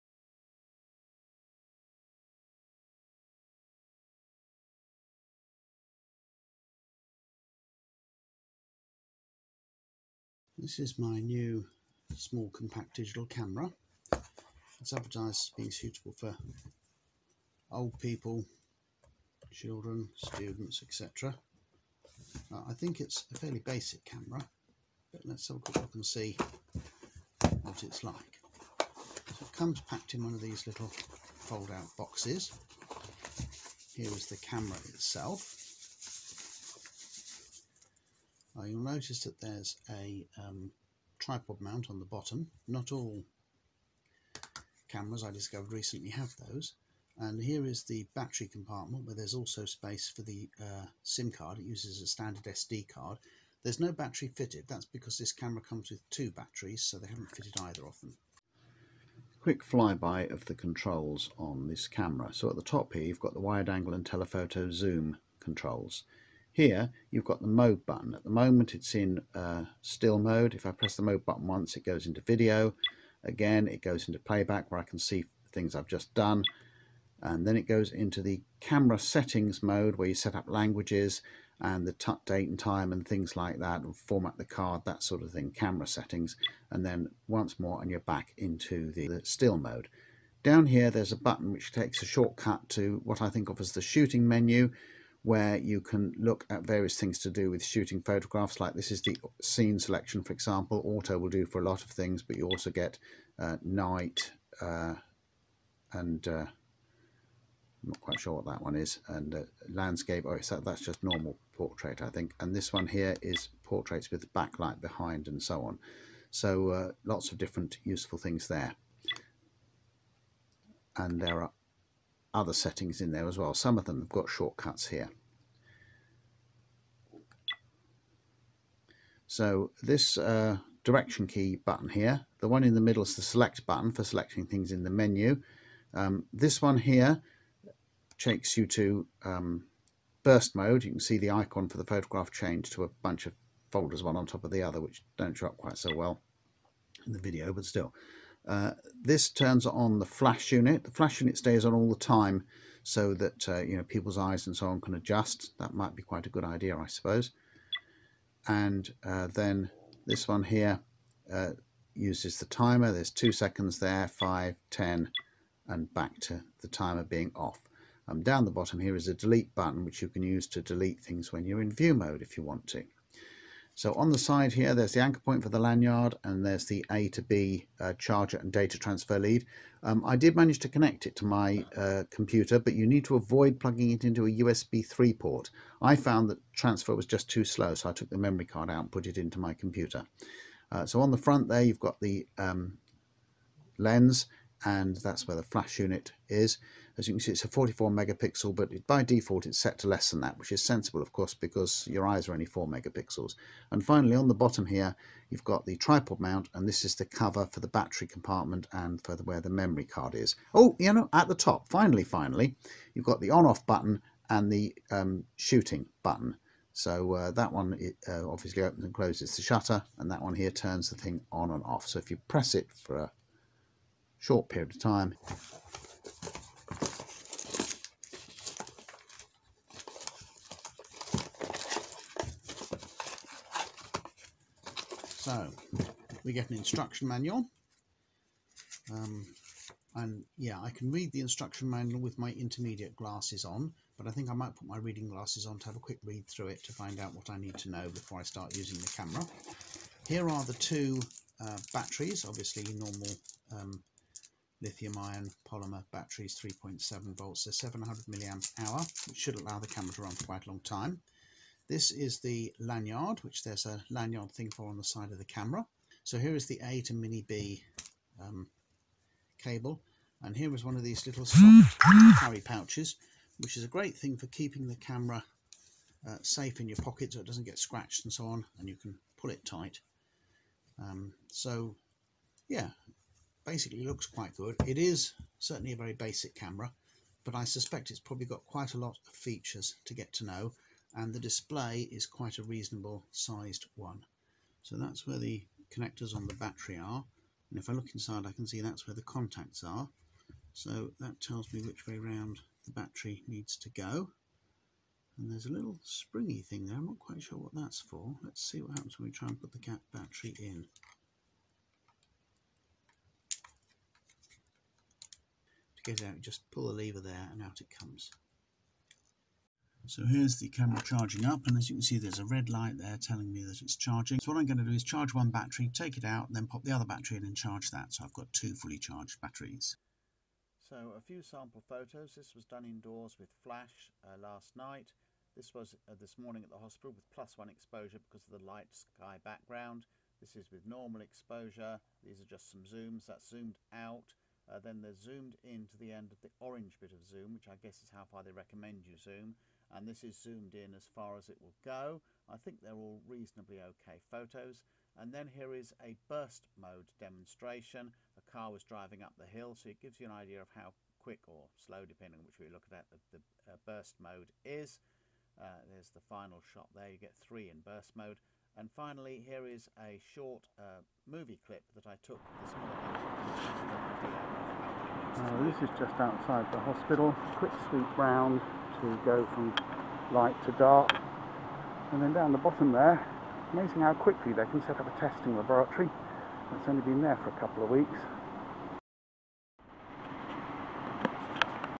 Vnieetser Camera
Another feature packed cheap compact camera.  Image quality is only OK, not many phone cameras will do better although access to different scenes, exposure settings, zooming etc on a phone can be a bit clunky.  This is great as a pocket camera and as a first device for older kids.